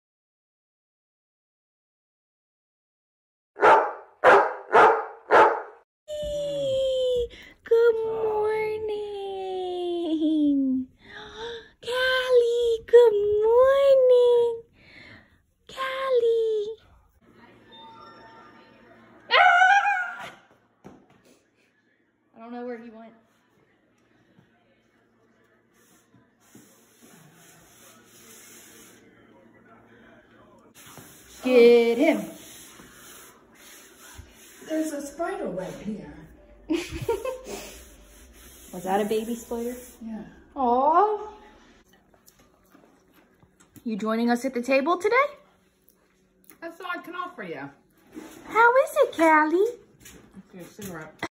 I just halp myself 🥘 - Day 369
Callie just helps herself to a plate at the table.